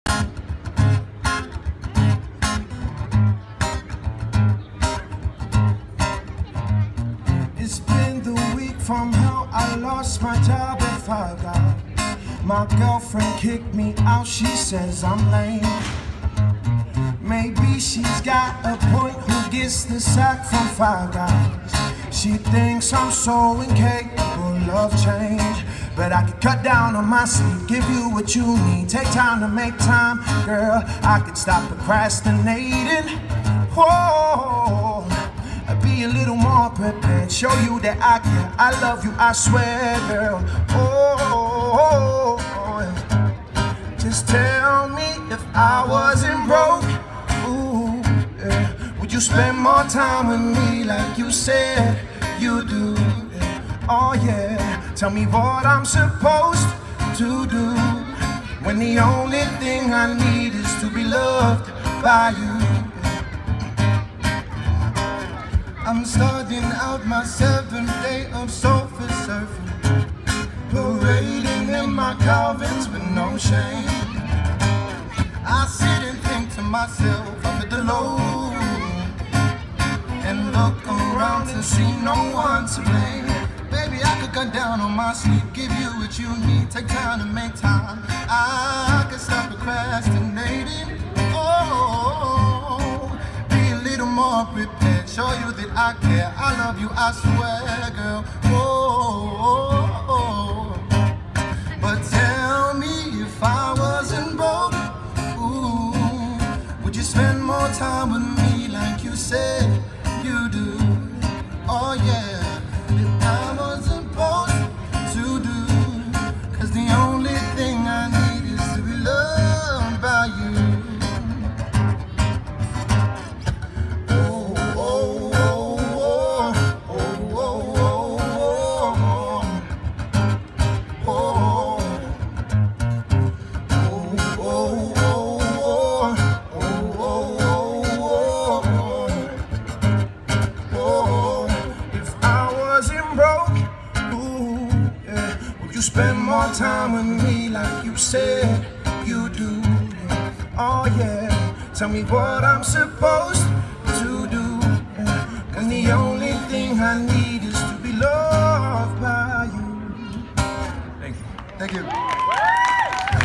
TANGAROA & NILE -live@Ahi Kā (Matariki) - Odlin's Plaza - Sat 18th July 2020
From Wellington, New Zealand comes singer/guitarists Tangaroa Teariki & Nile McGregor. Performing at the Maori New Year Matariki celebrations Ahi Kā. This was outdoors at the Odlin's Plaza on the waterfront in Wellington city. Last night Saturday 18th July 2020 a free concert held between 5-8pm

Tangaroa Teariki - Vocals/Guitar

Nile McGregor - Vocals